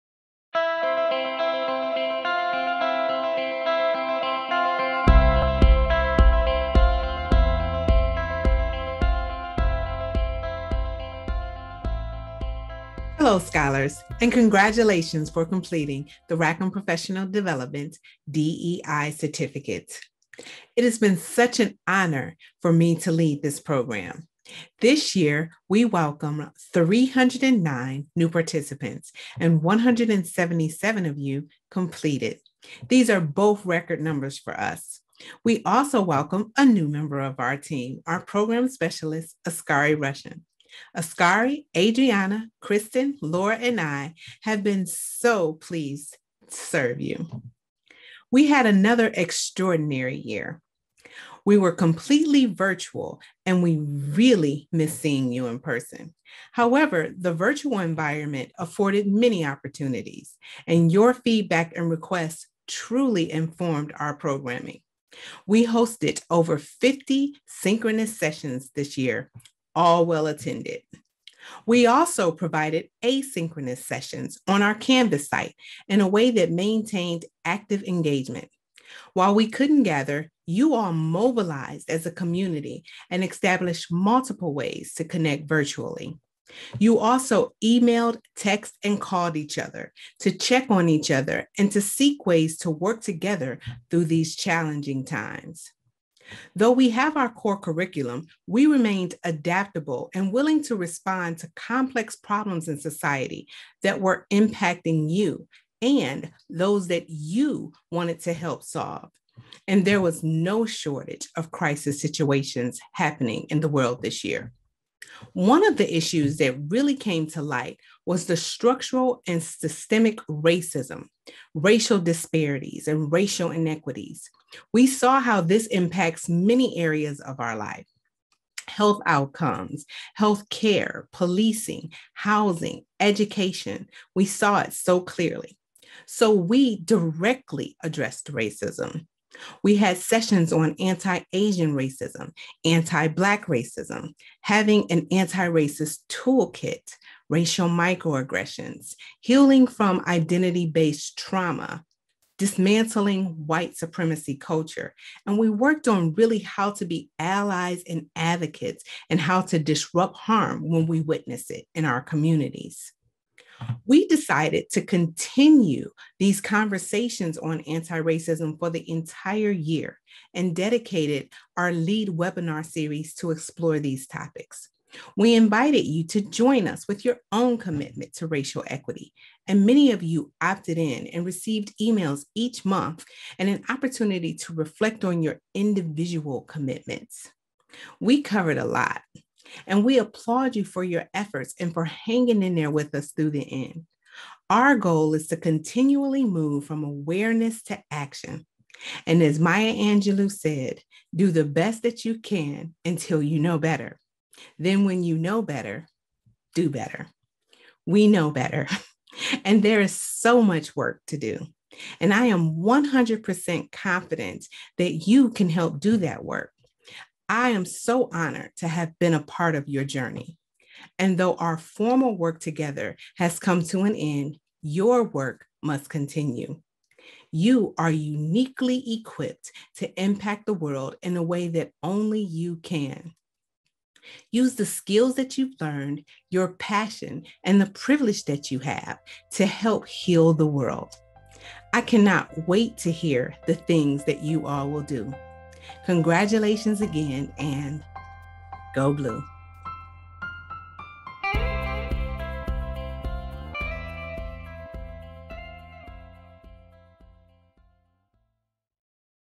2021 Rackham DEI Certificate: Remarks by Deborah Willis
Willis, who is Rackham Graduate School’s program lead senior, DEI certificate, offers remarks to the 2021 certificate recipients.